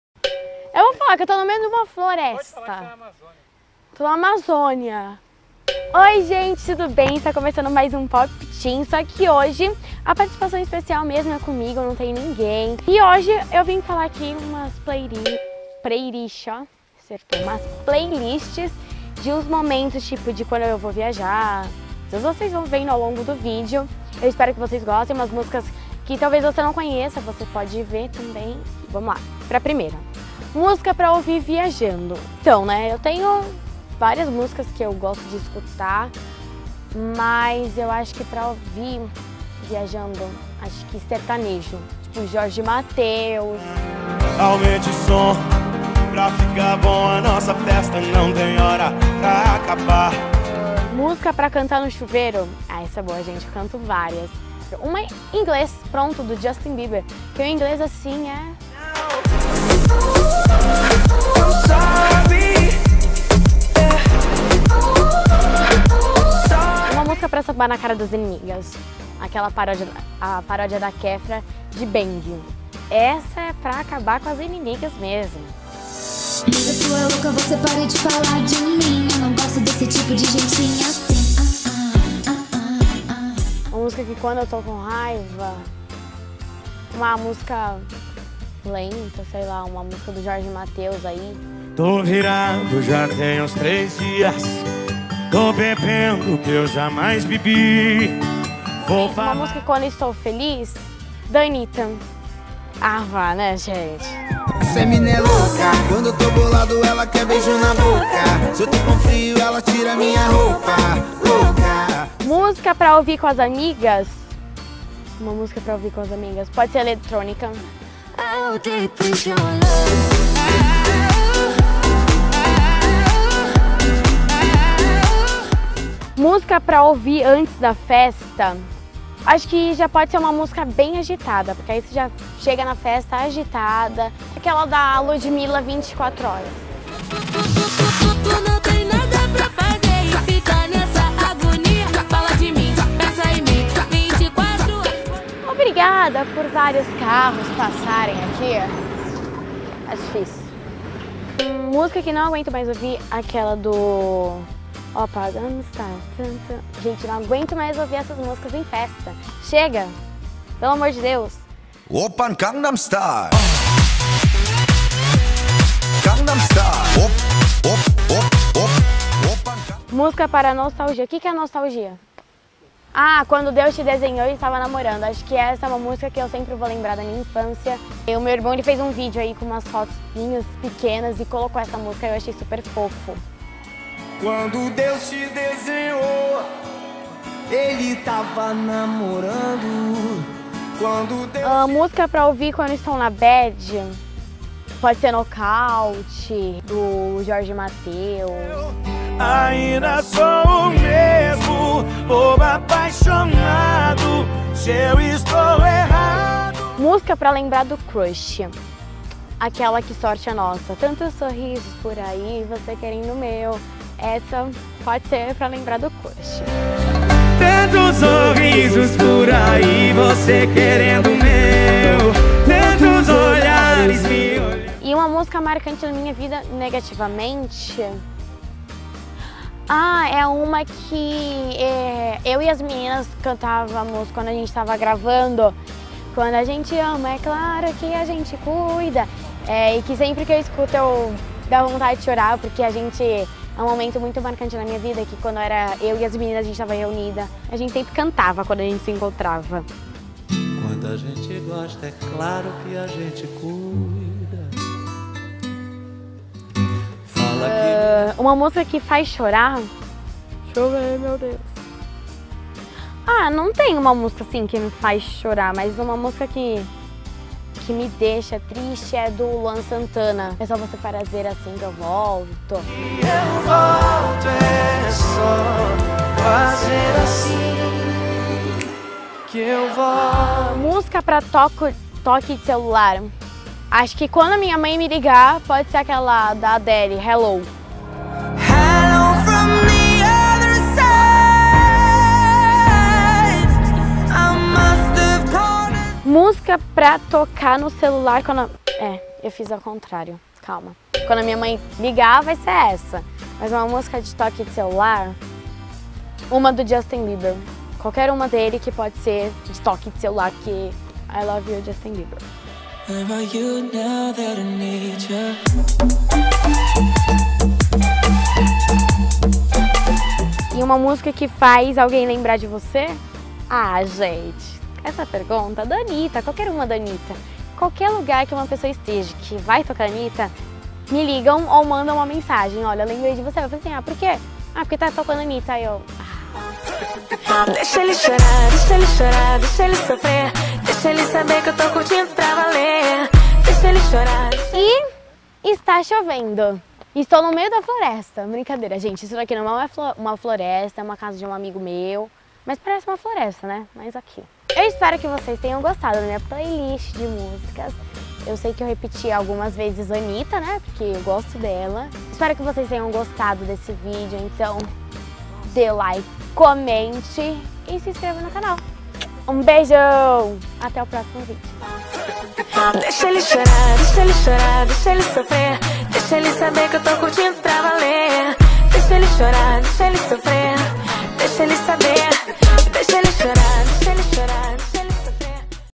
Tag: PLAYLIST DE MÚSICAS QUE ESCUTO NOS MOMENTOS - POP Teen
Neste vídeo você vai ver uma playlist de músicas que eu escuto em cada momento da minha vida. Veja se você gosta das mesmas músicas que eu.